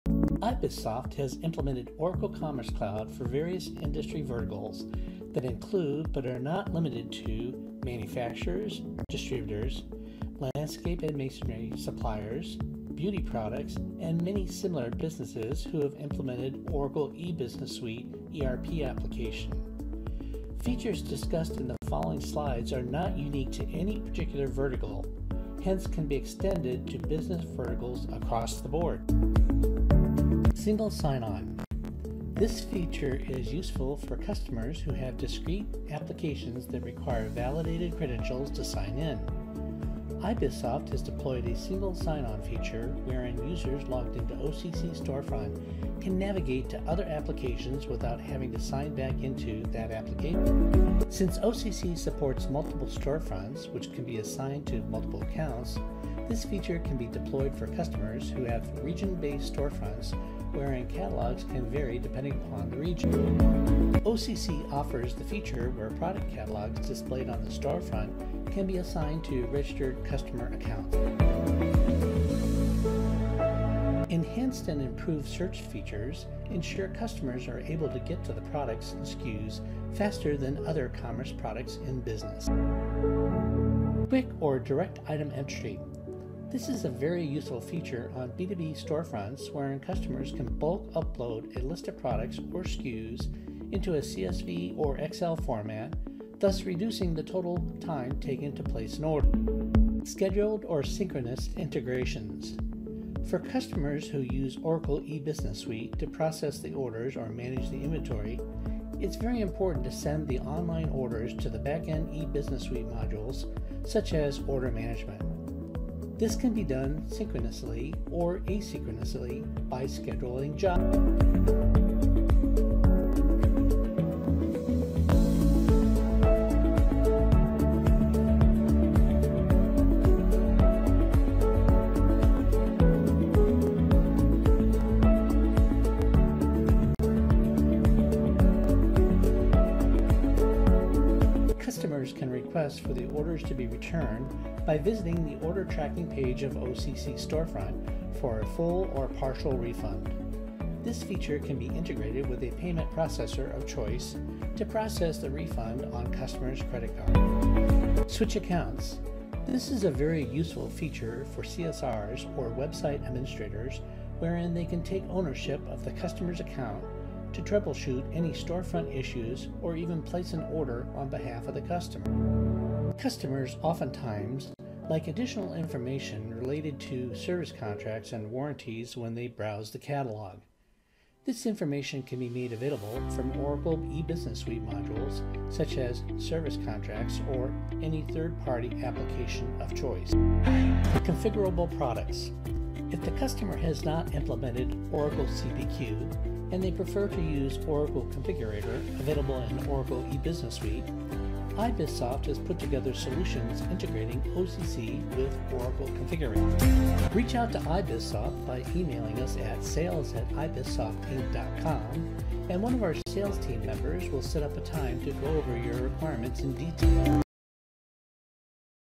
Oracle Commerce Cloud Features
iBizSoft has implemented OracleCommerceCloud for various industry verticals that include but not limited to Manufacturers, Distributors, Hardscape & Masonry suppliers, Beauty Products and many similar businesses who have implemented Oracle E-Business Suite ERP application. Features discussed in the following slides are not unique to any particular vertical hence can be extended to business verticals across the board.